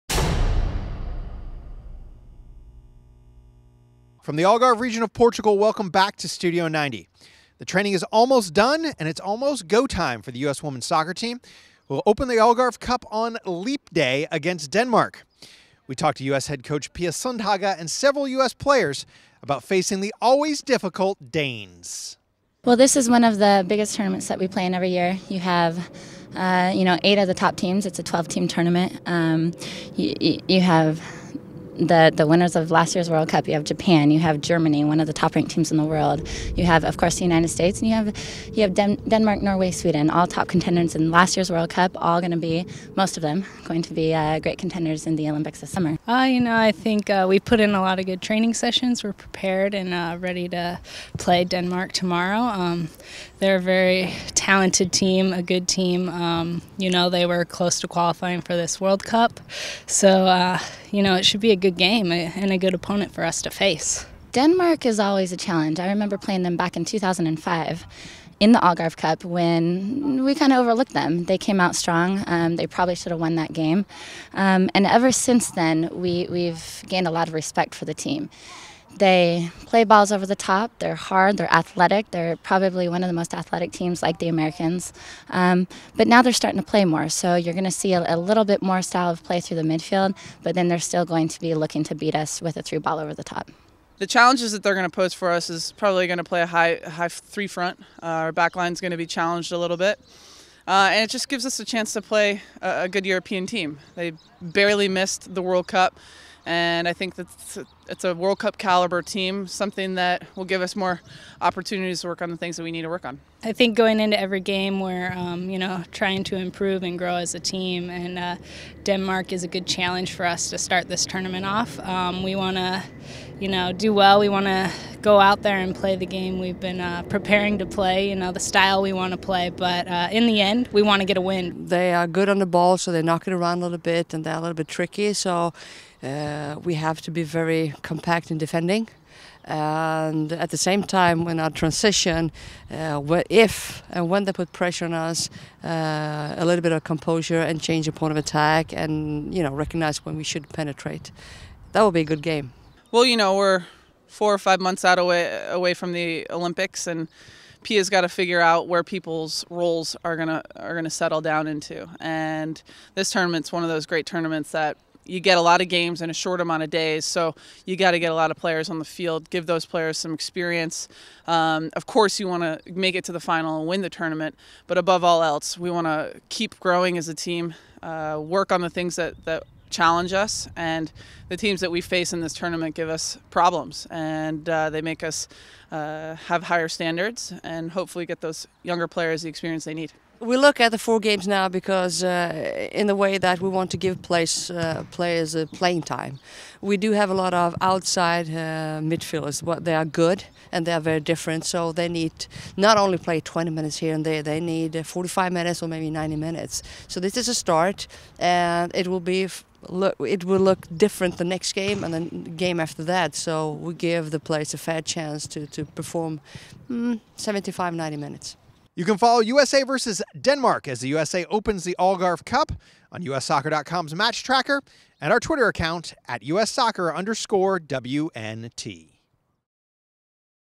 Studio 90: WNT Set to Open Algarve Cup on Leap Day against Denmark
U.S. head coach Pia Sundhage and players preview the "Leap Day" opener vs. Denmark at the 2012 Algarve Cup